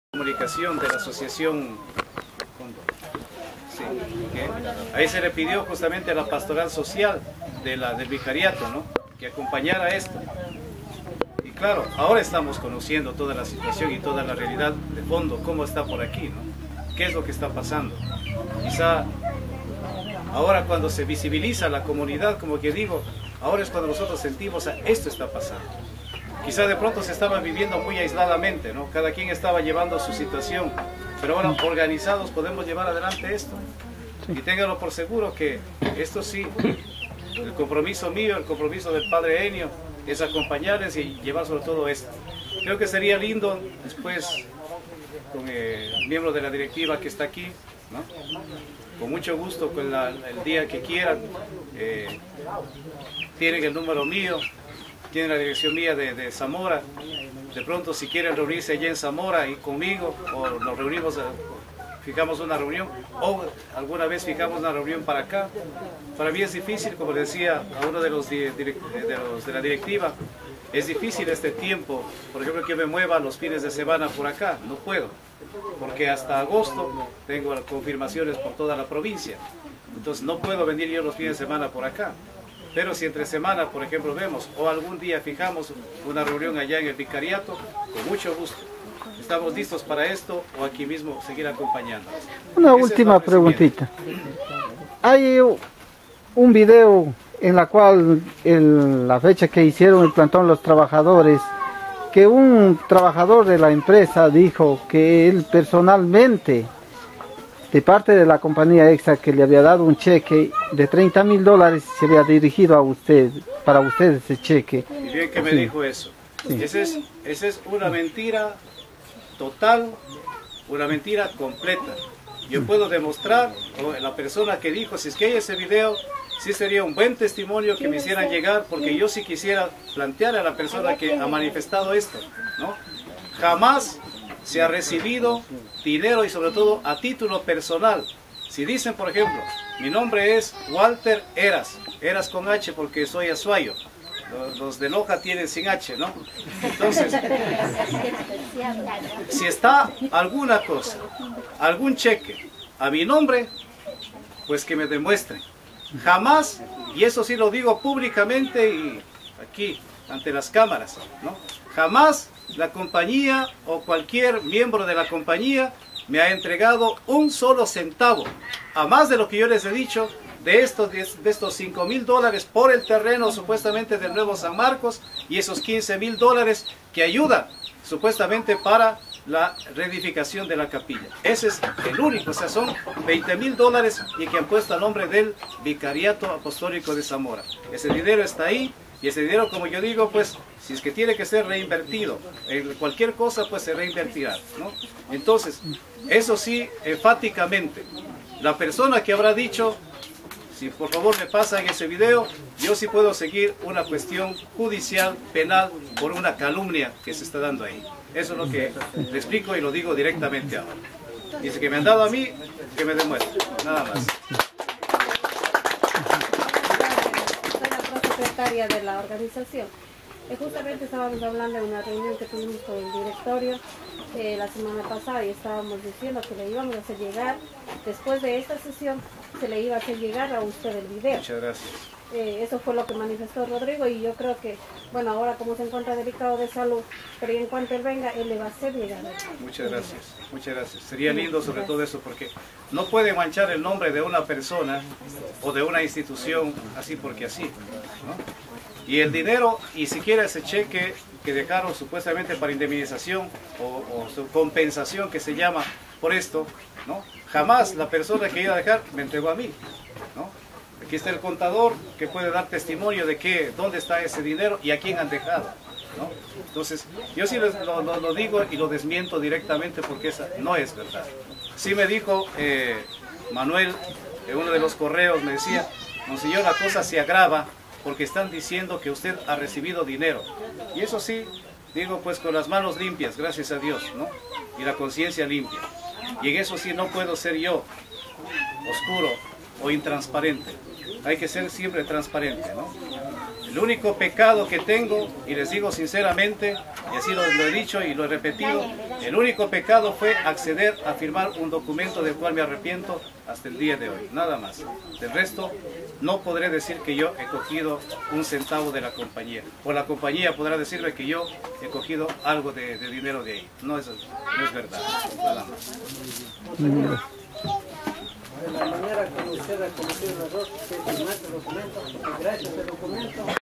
PARTE 2 DECLARACION MON HERAS CASO SAN MARCOS
MONS. WALTER HERAS VICARIATO DECLARA PUBLICAMENTE CASO SAN MARCOS-CORDILLERA DEL CONDOR.